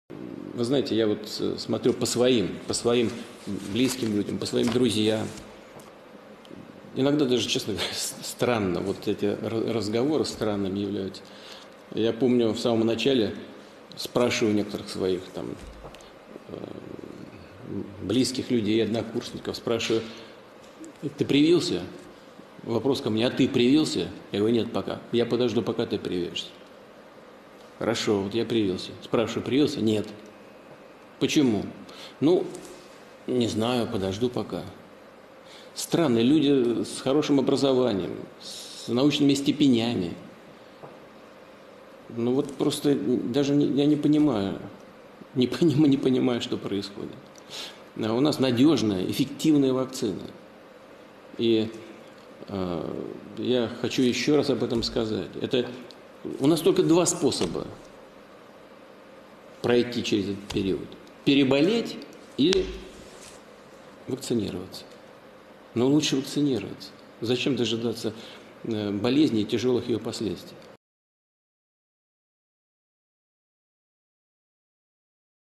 Hoy comentamos sobre la vacuna contra el Covid, Vladimir Putin comenta
Presidente de Rusia Vladimir Putin comenta sobre vacuna contra el Covid.